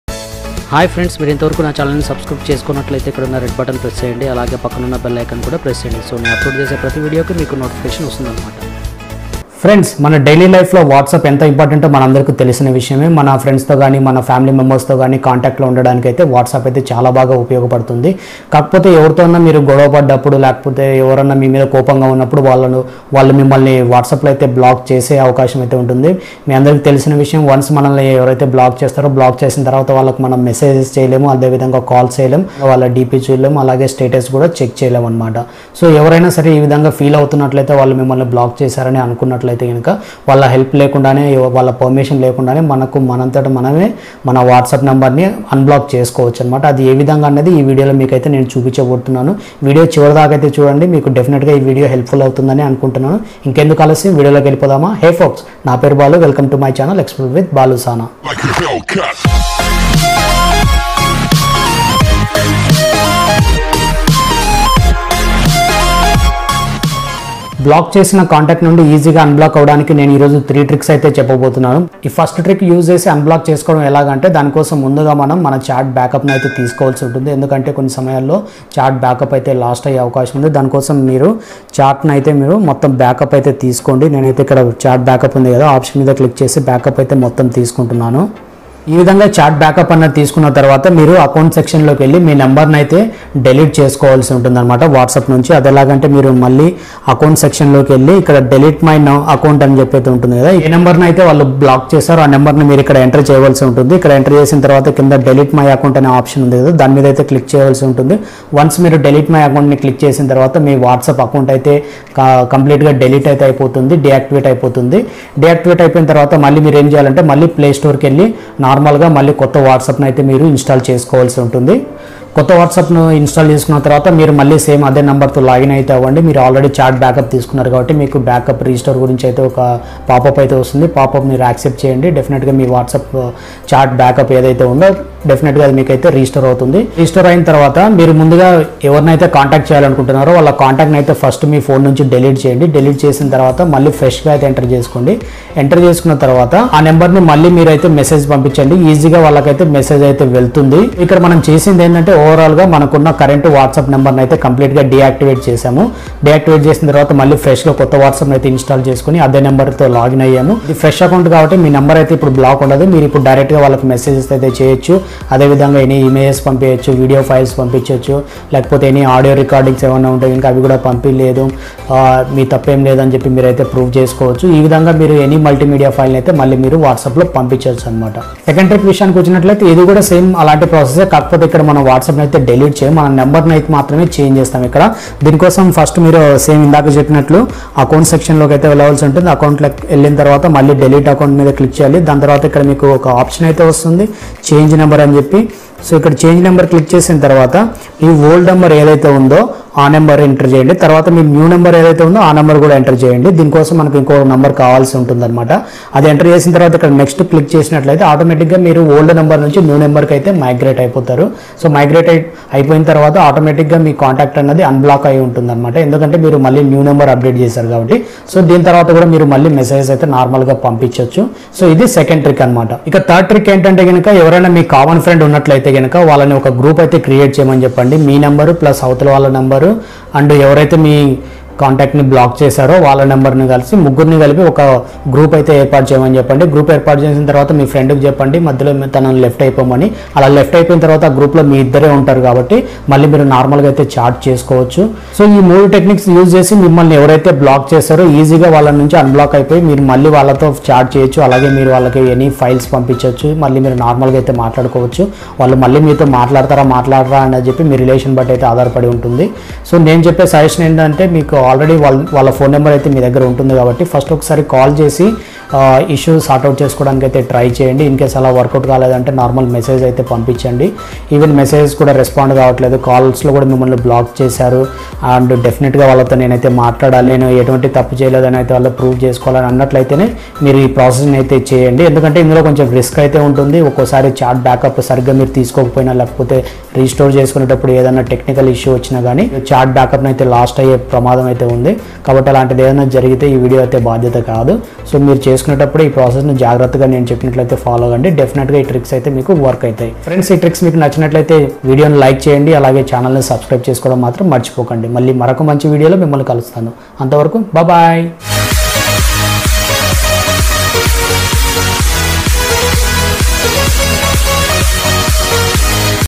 How to Message a person who blocked you on WhatsApp II In Telugu by Explore with BaluSana II
Hi Friends, in this video I have explained the tricks of how to unblock yourself and send messages to the person who blocked you on WhatsApp.